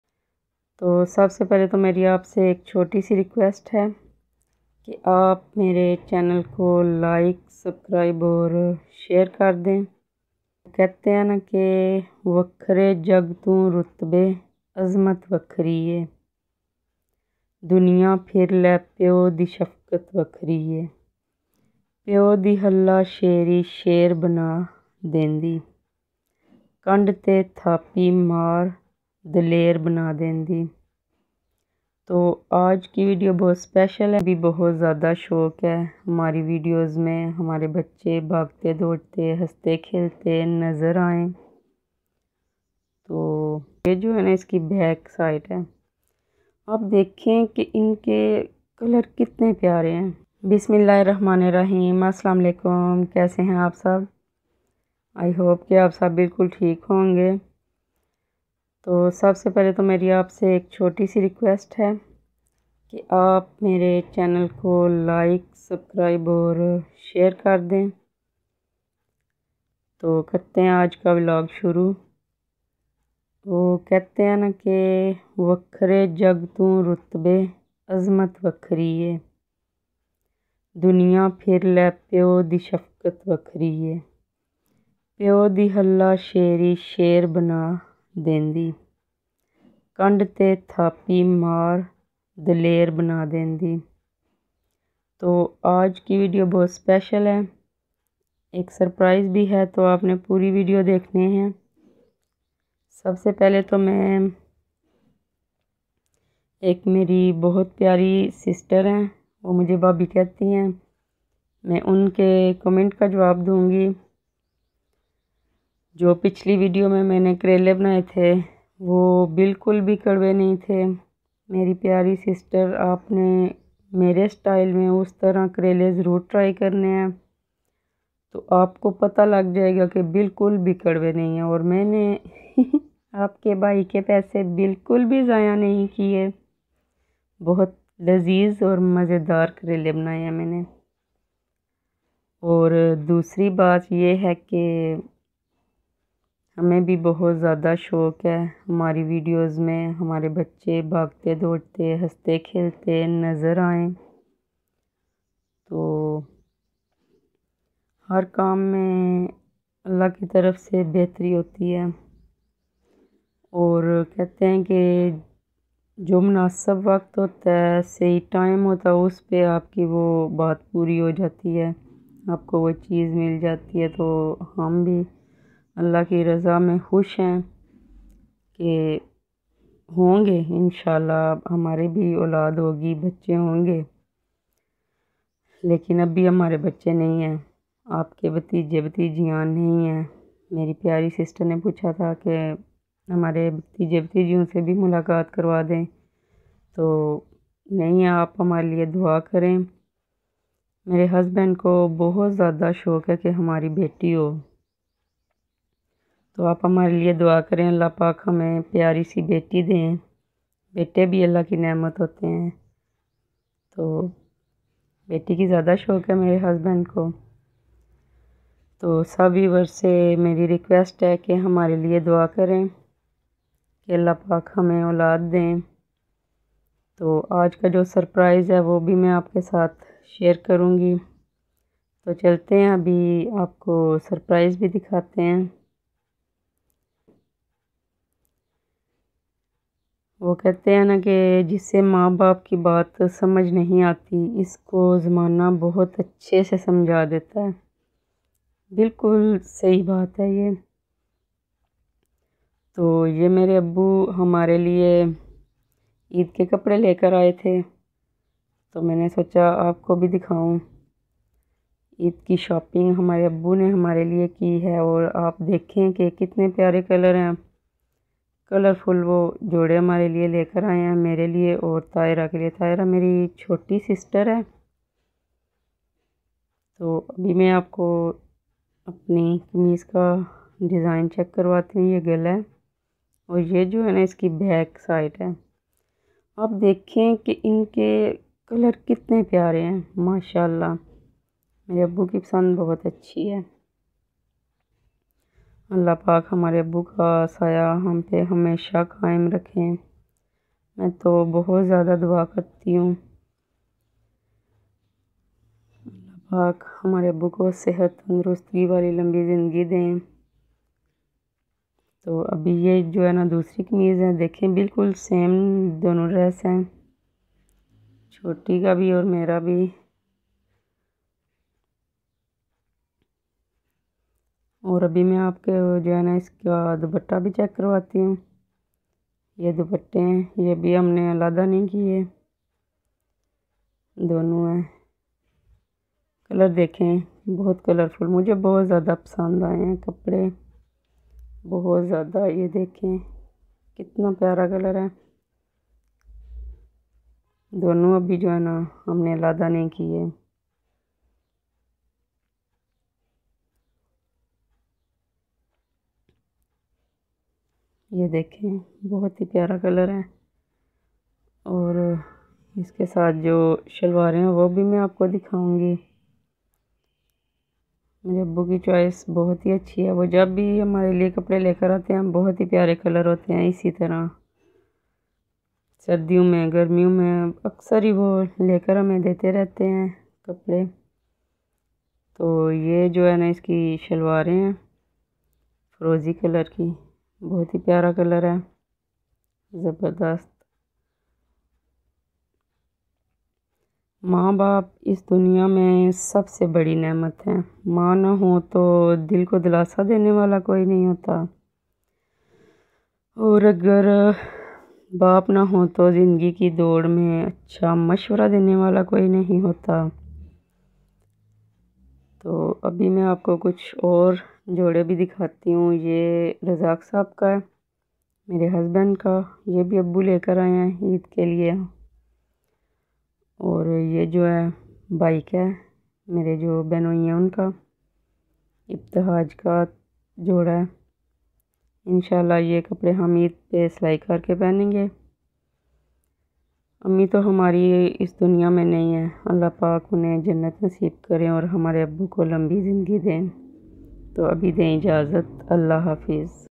Aj hmy kis ne surprise dia ap b dekhen | Apni bot pyari sis k comment ka jwab | Sitara Razzaq vlog
Aslamoalikum me hun Sitara Razzaq me lai hun ap k liye boht pyara vlog umeed hy ap ko psand ay ga